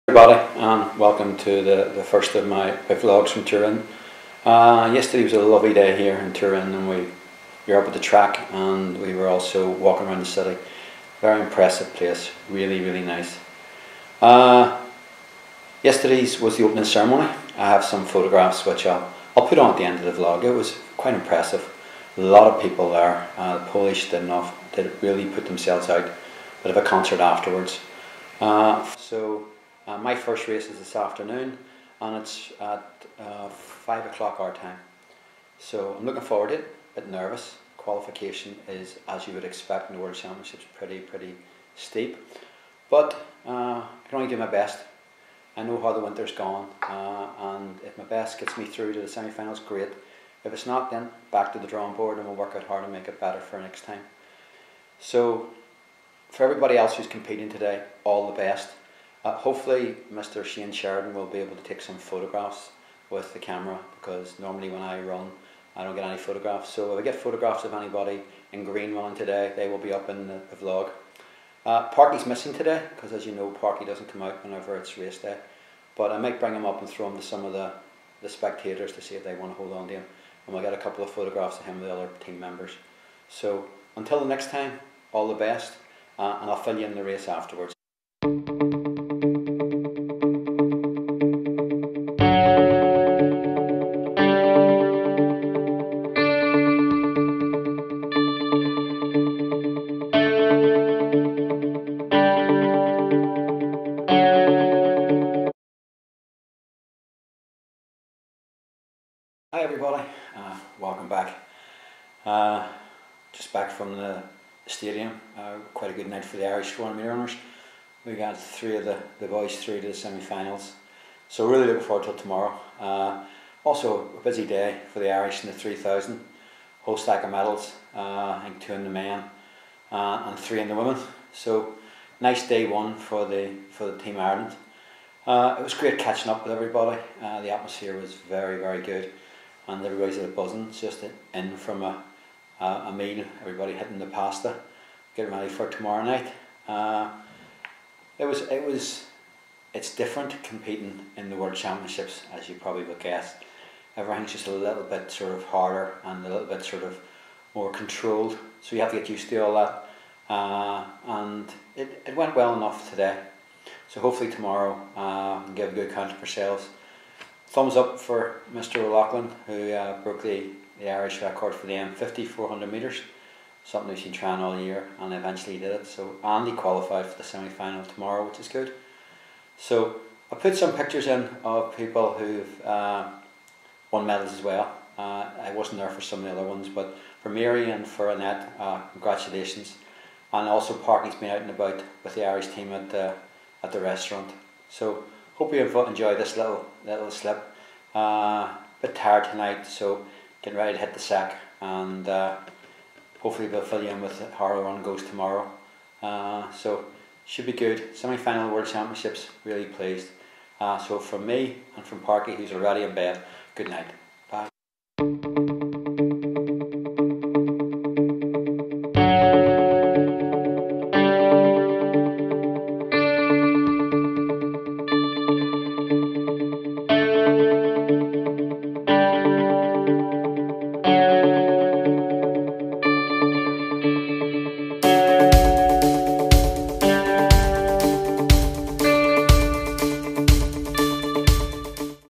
Parky and I hit Torun
This video is about arriving in Torun and first Race in World Indoor Champs. Hope you enjoy.